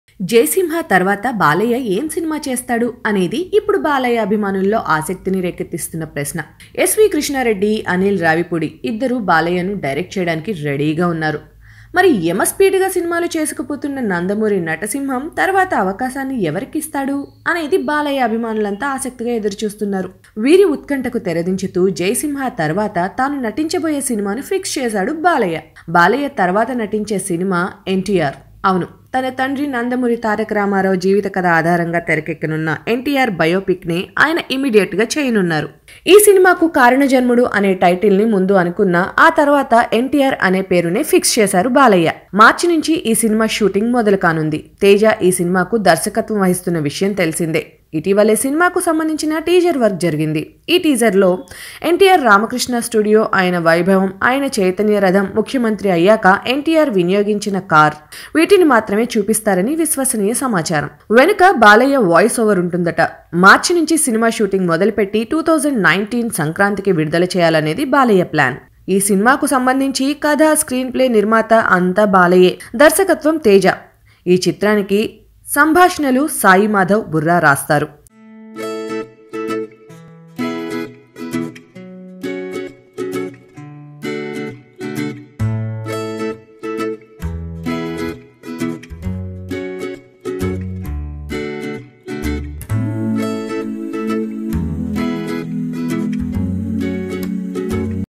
Balakrishna Next Movie Confirmed
The Biopic of Sr NTR Nandamuri Balakrishna who is playing the role of his legendary father, has recently started shooting for the teaser in Ramakrishna Studios in hyderabad.

This Is What Prabhas Doing For New Year 2018 -